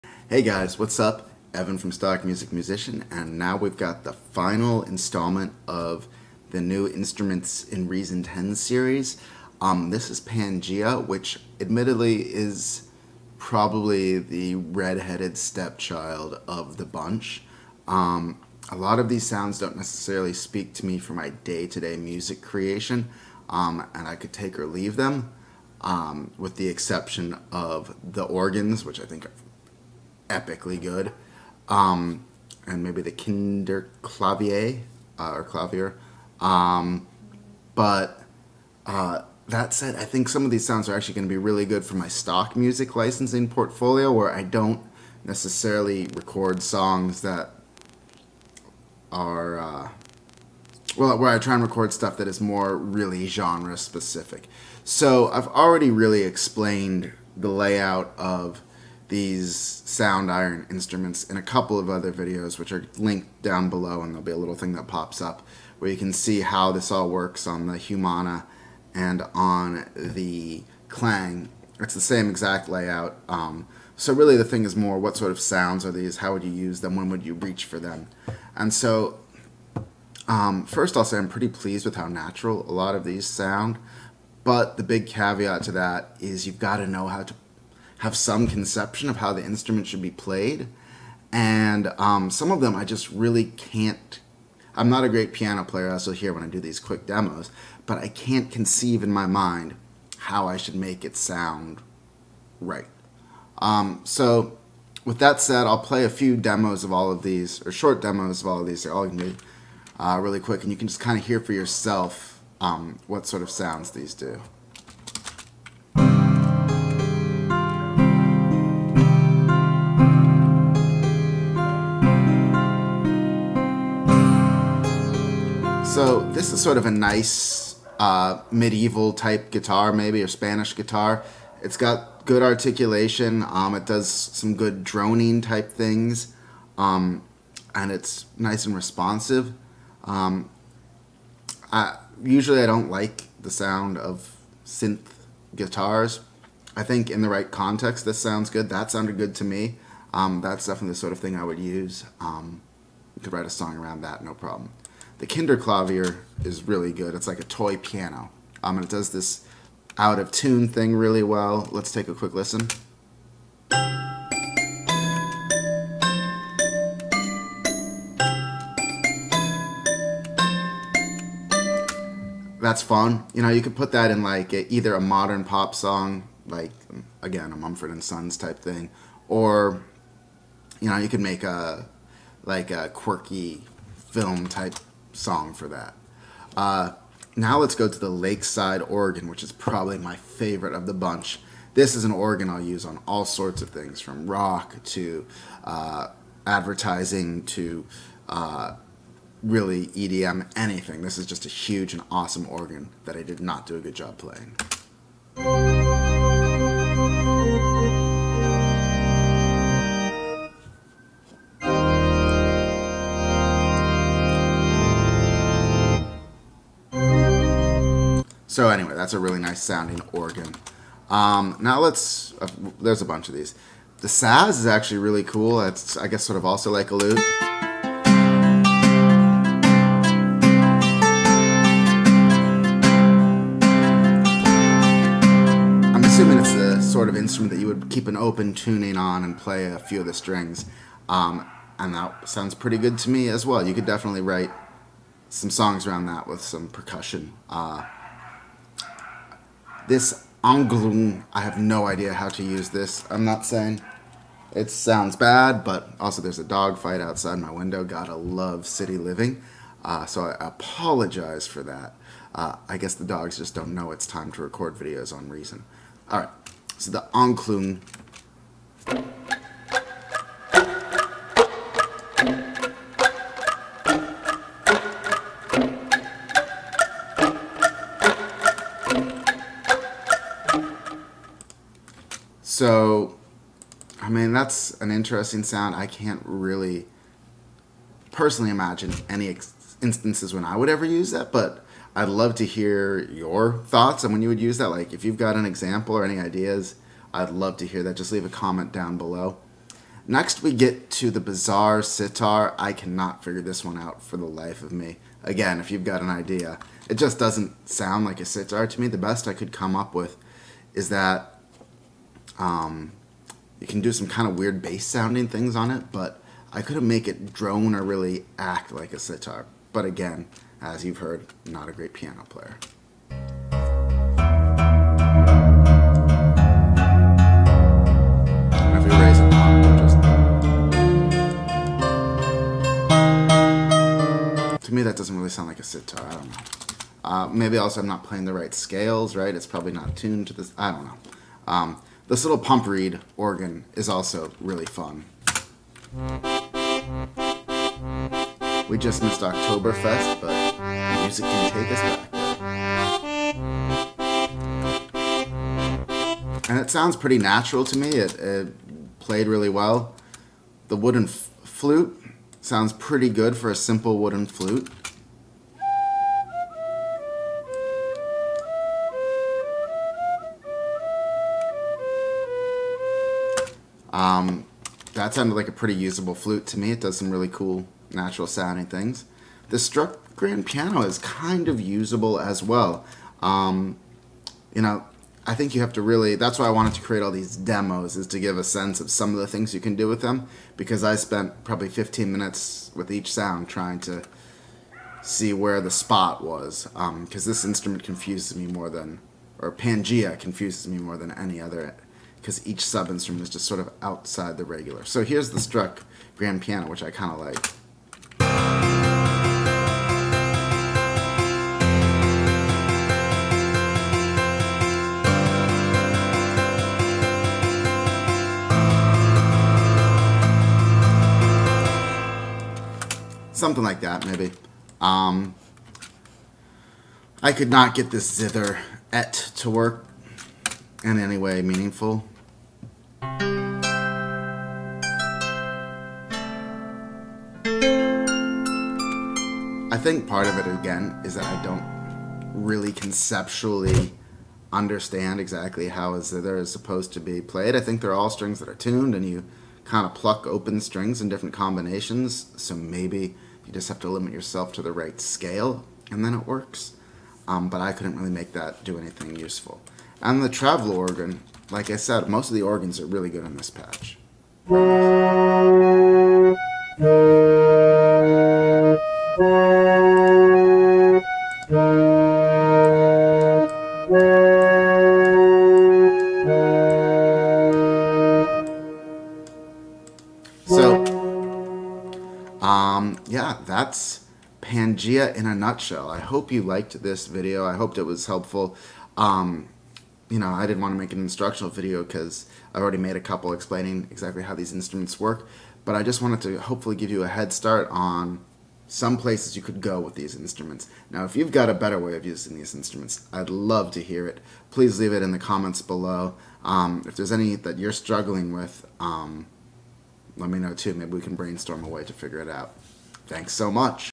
Reason 10 Pangea Demos and Overview
This Reason 10 Pangea demo shows off some of the new sounds in Reason 10. I don't think I'll use Pangea too much, but it's nice to have. And the organs are fantastic.But some of the Pangea demos I just can't figure out how to use.

The interface for Pangea follows that layout I've already discussed in my Klang overview and my Humana overview

Propellerhead Software's Reason 10 adds a host of new features.The new Sound Iron instruments may be easily overlooked, but they add an authentic, realistic layer of sound to your productions.